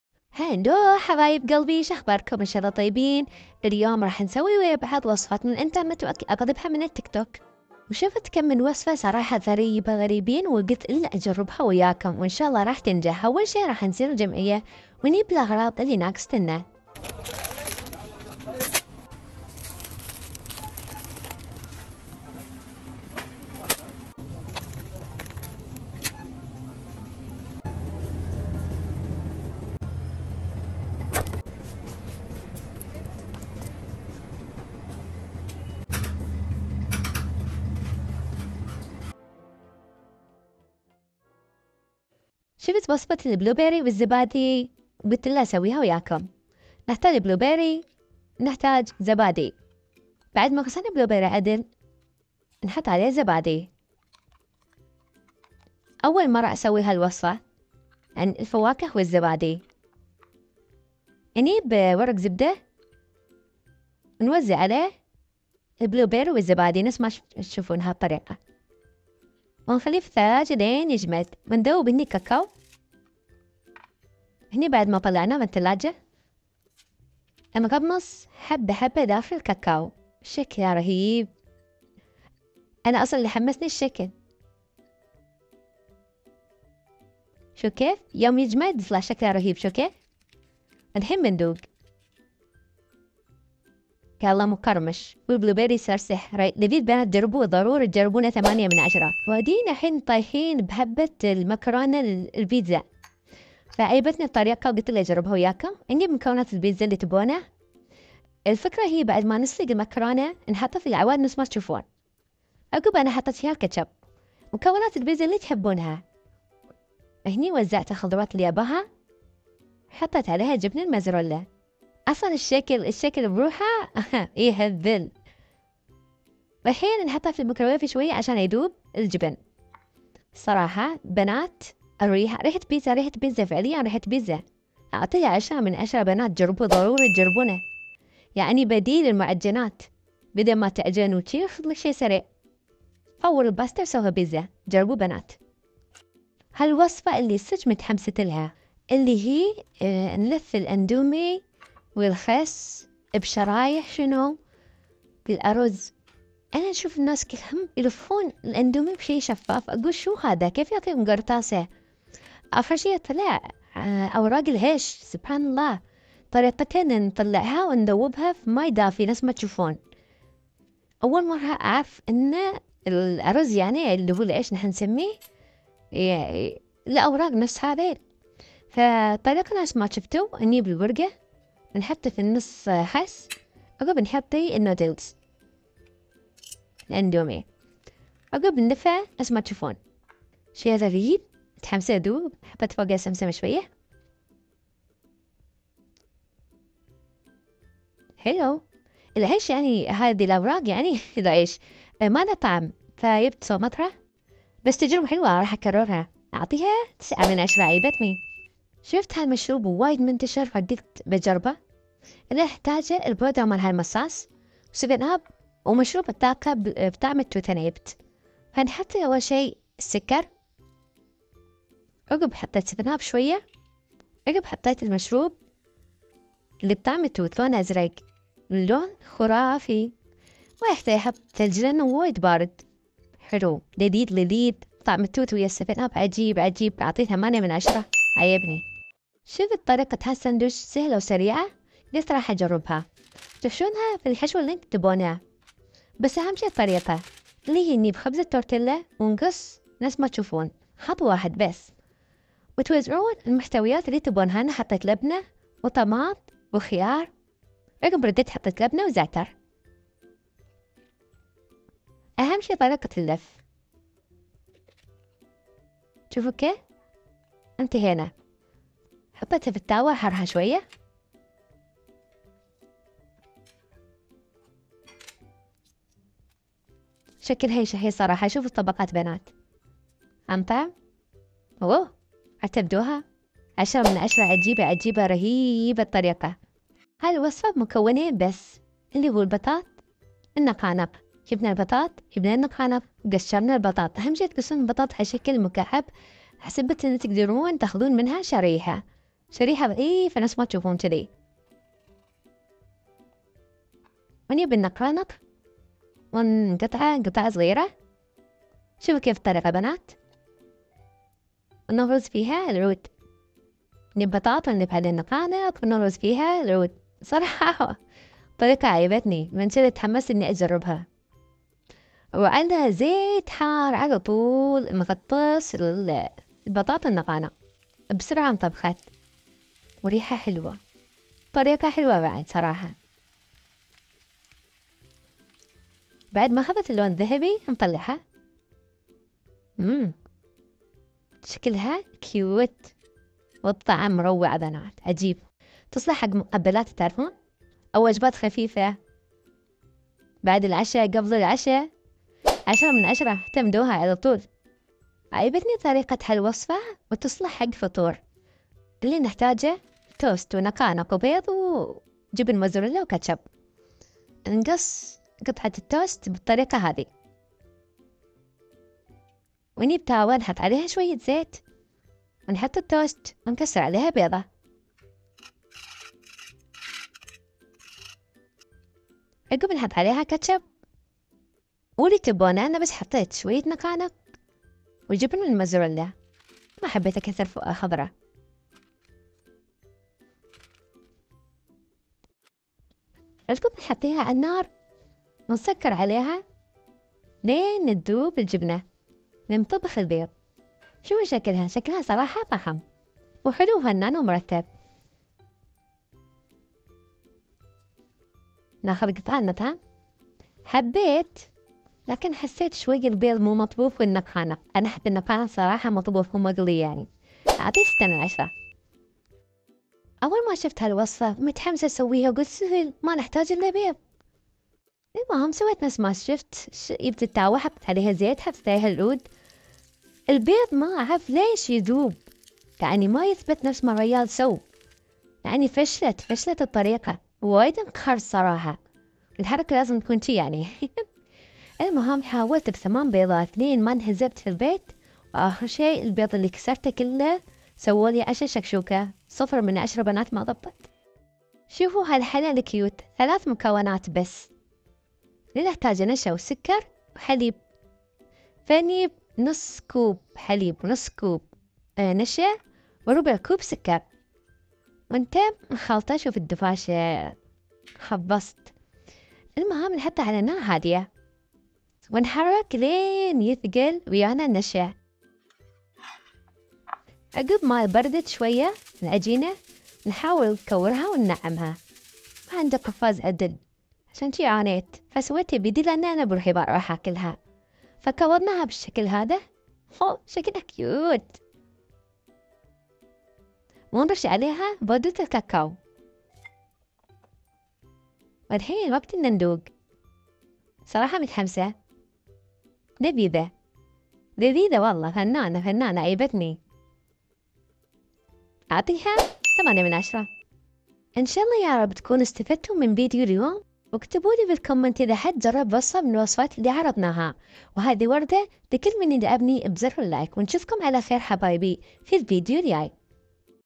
جربت آخر هبات الانترنت 🙄| سويت رولات الاندومي🥵
جربت آخر هبات الانترنت

أنا اميرة
أشارككم بساطة تجارب و ترندات الأكلات بطريقة مشوقة و ممتعة
أريد قضاء وقت ممتع و هادئ معكم